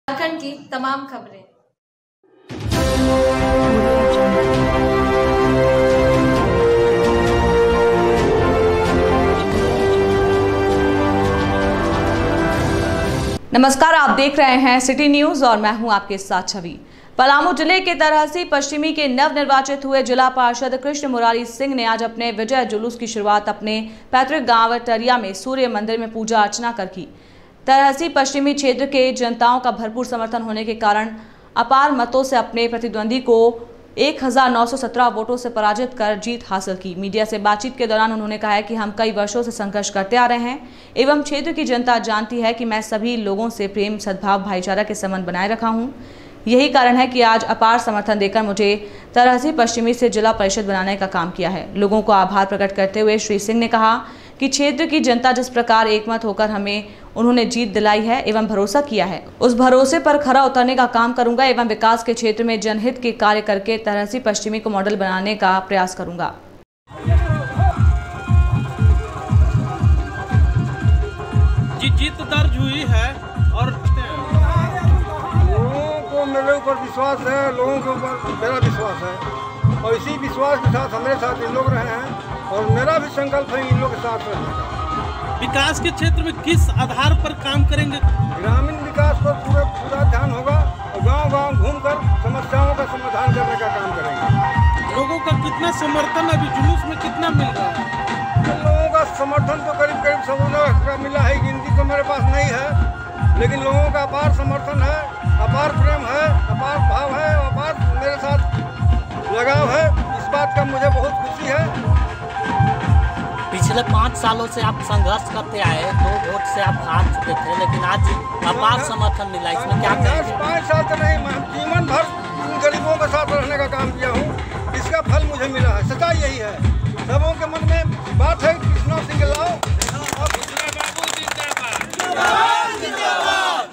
विकास के क्षेत्र में मॉडल के रूप में तैयार होगा तरहसी पश्चिमी :- कृष्ण मुरारी
तरहसी से सुधाकर पांडे की रिपोर्ट//jharkhand bihar news